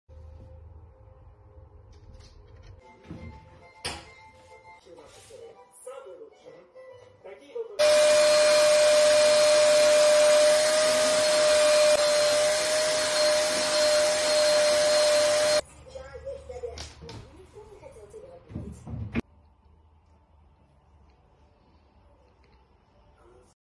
When the cat defeats the rack: a day in the life of vacuuming sawdust from a mischievous feline
A cat owner from Nizhny Novgorod, Russia captured a funny moment of her cat Royce defying a barrier and making a cozy bed in sawdust. @moth.nail.nn shared the video on her profile on March 31st 2023, and it quickly gained attention from fellow cat lovers and humor enthusiasts.

According to the author, her husband had built a rack to block the cat's access to the rabbit house, which is apparently a spot that the feline likes to lounge on. However, the cat proved to be a determined escape artist and managed to crawl through the gaps and onto the sawdust that had been scattered on the rabbit house for bedding.

The author found the sight of her cat sleeping in the sawdust hilarious and decided to film it while cleaning up the fur with a vacuum cleaner. The video shows the cat lying on its side, purring contentedly, as the girl gently sucks the sawdust from its fluffy coat. The cat looks up at her a few times but doesn't seem bothered by the vacuum's noise or suction.

Location: Russia